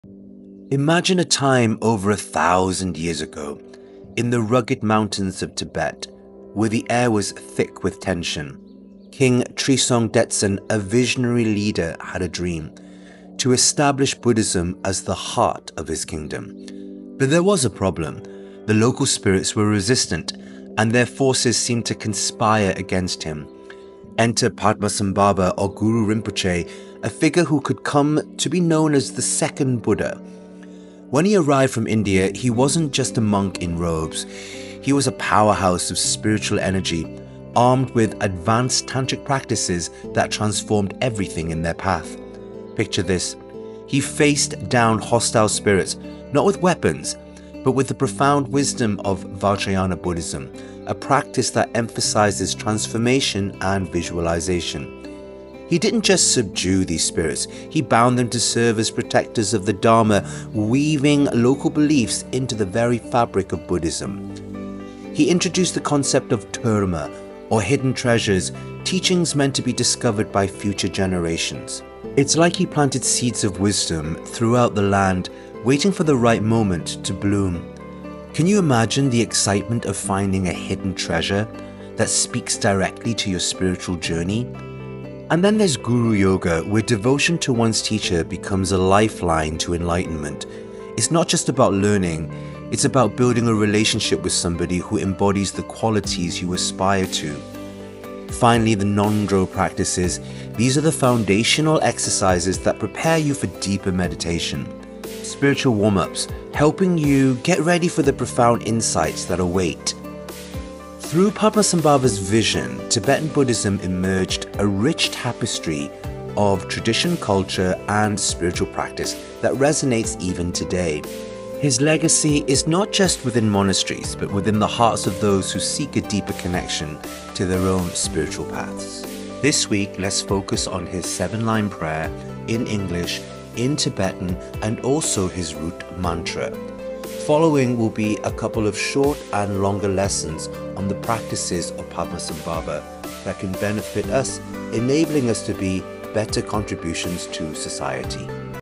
How Padmasambhava’s Arrival Changed Tibet Forever: The Birth of Vajrayana Buddhism!
Discover the extraordinary origins of Padmasambhava, the legendary “Second Buddha” who transformed Tibetan Buddhism. In this video, we explore why Padmasambhava was invited to Tibet by King Trisong Detsen, his mission to overcome powerful obstacles, and how he introduced Vajrayana Buddhism, blending local beliefs with profound tantric practices.

Learn how Padmasambhava established the foundation of Tibetan Buddhism, integrating teachings that still resonate today: the transformative power of tantra, the importance of Guru Yoga, and the unique terma tradition of hidden teachings. This is the story of how Padmasambhava’s presence continues to shape Tibetan spirituality and the practice of Vajrayana Buddhism around the world. Join us to understand the timeless impact of this revered master and his contributions to Buddhism.